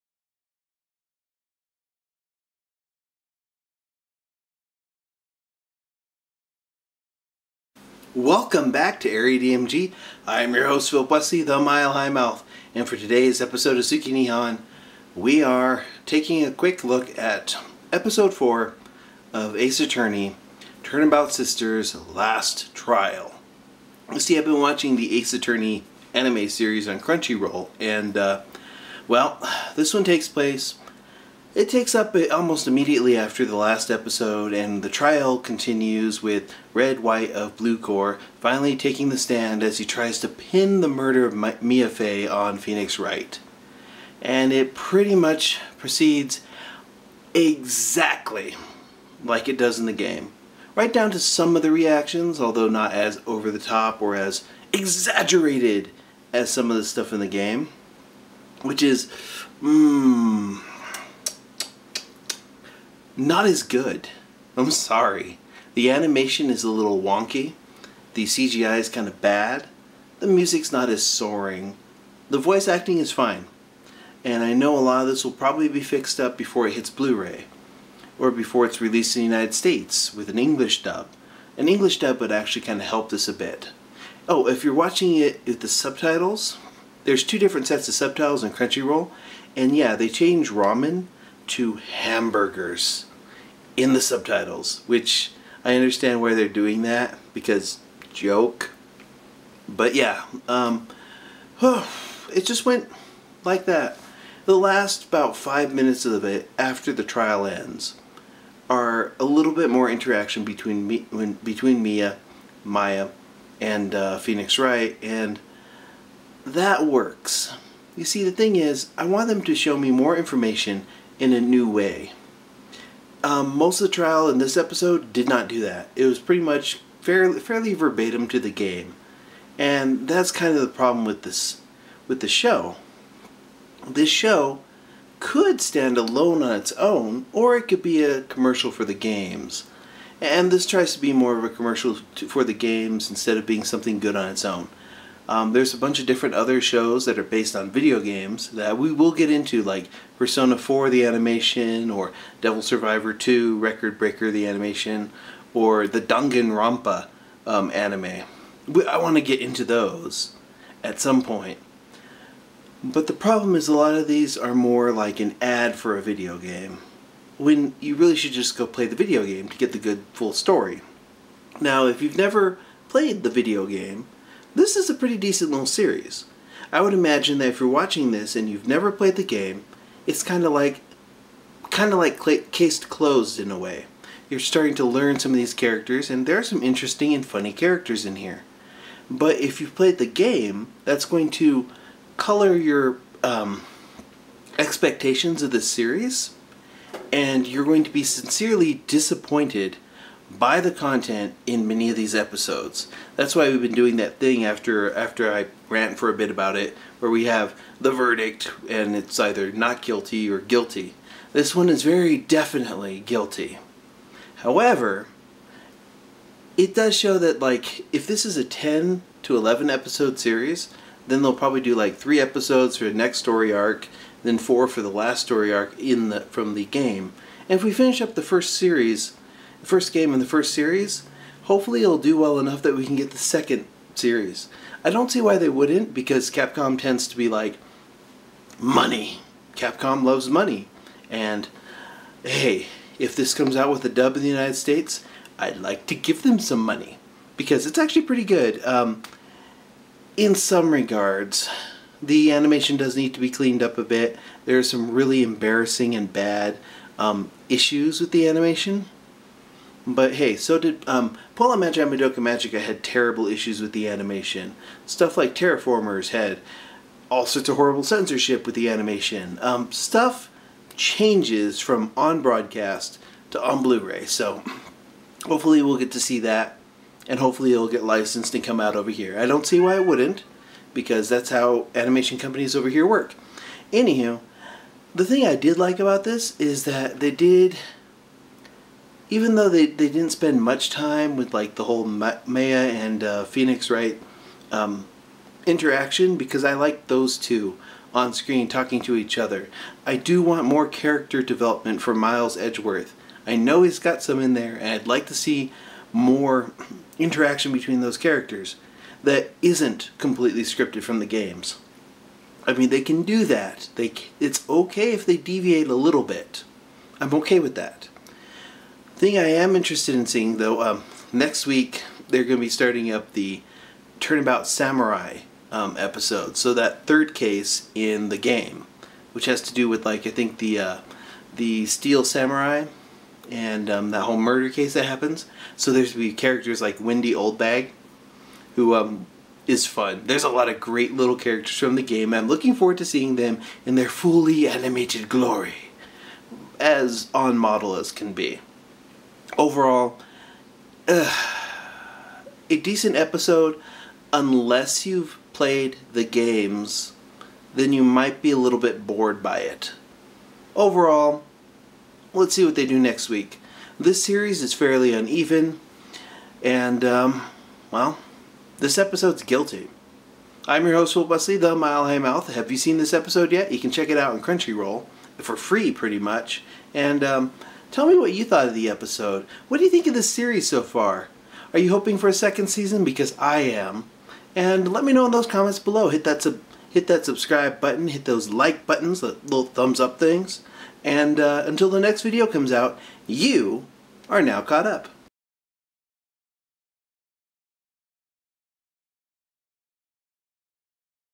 Ace Attorney - Turnabout Sisters: Last Trial - Review/Discussion - 104 - SUB
In This Video: The last part of the trial is animated and.. I'm only kind of okay with it.